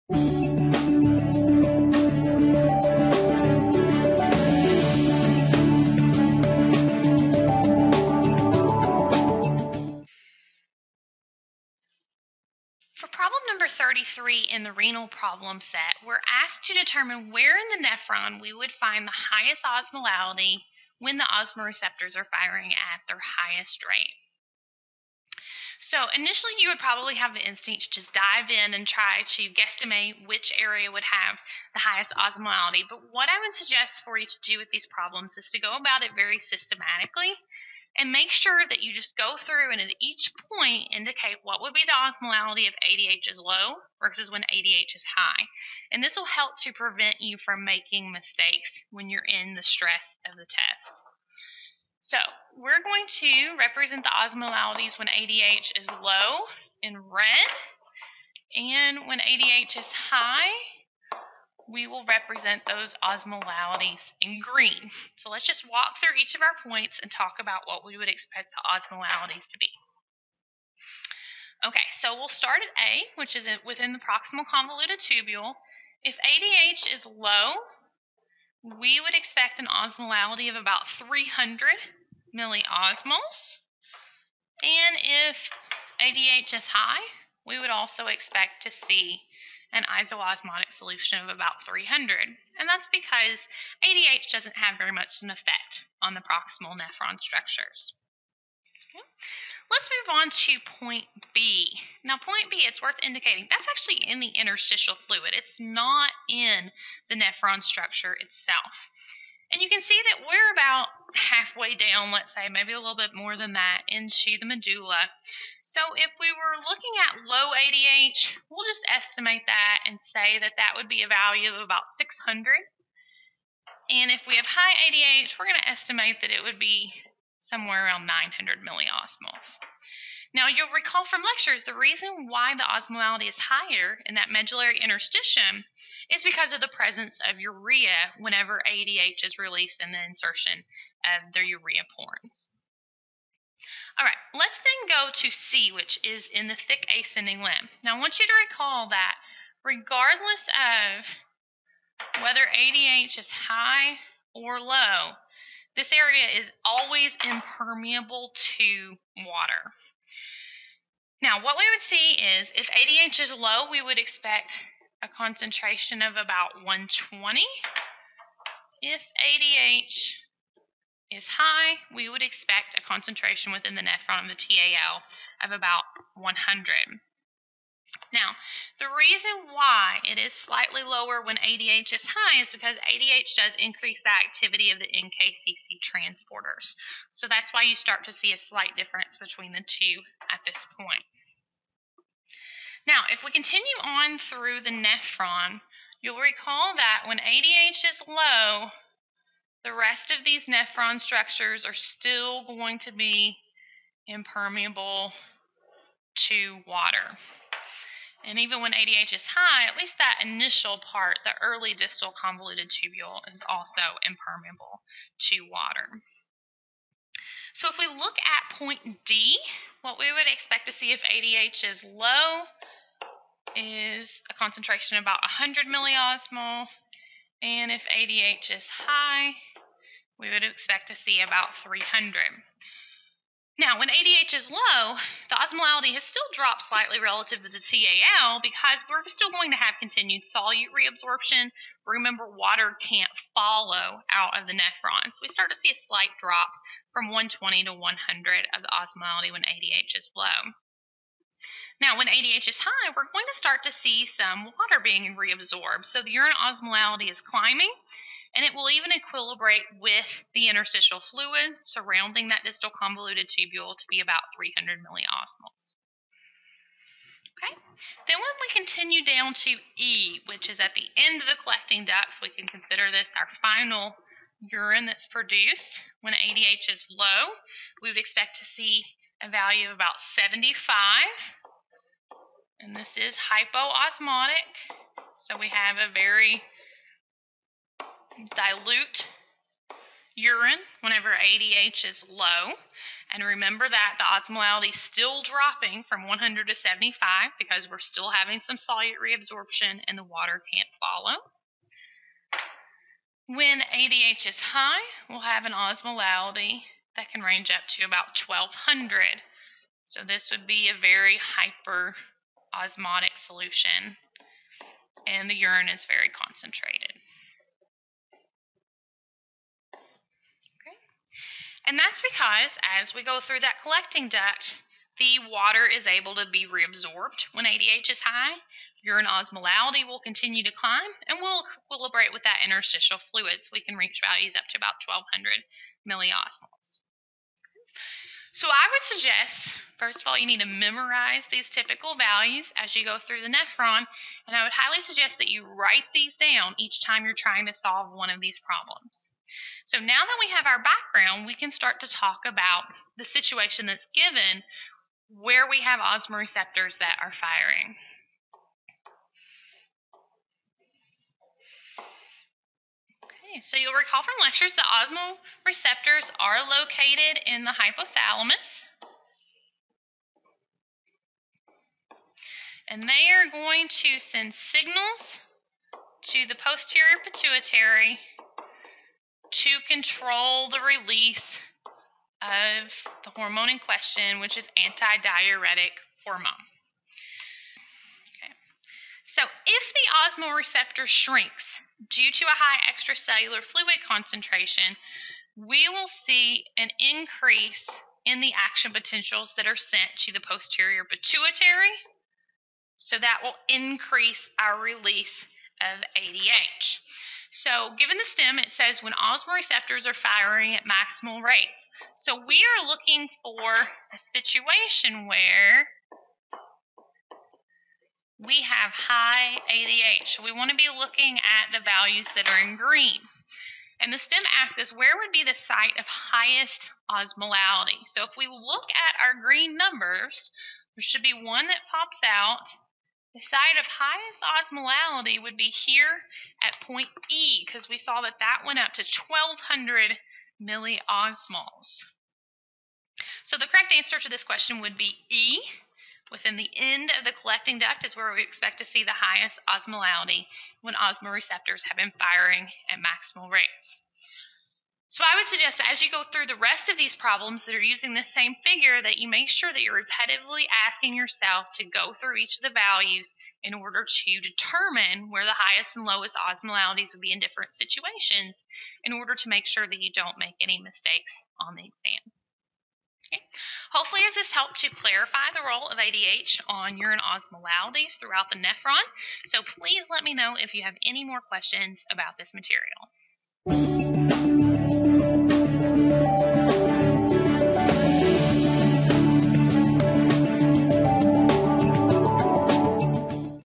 Renal Physiology #33
In this video, Dr. Cynthia Miller solves #33 in the Renal Physiology problem set.